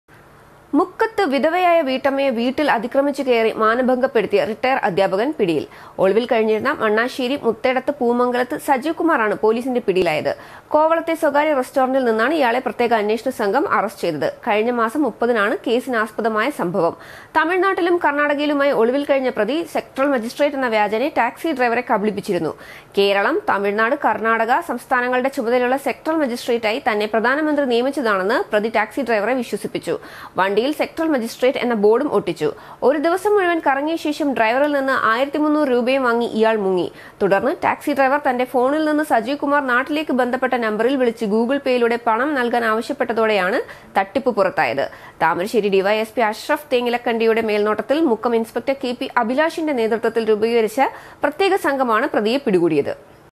മുക്കത്ത് വീട്ടമ്മയെ പീഡിപ്പിച്ച റിട്ട. അദ്ധ്യാപകന്‍ പിടിയില്‍ Man arrested for molesting housewife
മുക്കത്ത് വിധവയായ വീട്ടമ്മയെ വീട്ടില്‍ അതിക്രമിച്ച് കയറി മാനഭംഗപ്പെടുത്തിയ റിട്ട.അദ്ധ്യാപകന്‍ പിടിയില്‍. ഒളിവില്‍ കഴിഞ്ഞിരുന്ന മണാശ്ശേരി മുത്തേടത്ത് പൂമംഗലത്ത് സജീവ് കുമാറാണ് പൊലീസിന്റെ പിടിയിലായത്. കോവളത്തെ സ്വകാര്യ റസ്ടോറന്റില്‍ നിന്നാണ് ഇയാളെ പ്രത്യേക അന്വേഷണ സംഘം അറസ്റ്റ് ചെയ്തത്. കഴിഞ്ഞമാസം 30നാണ് കേസിനാസ്പദമായ സംഭവം.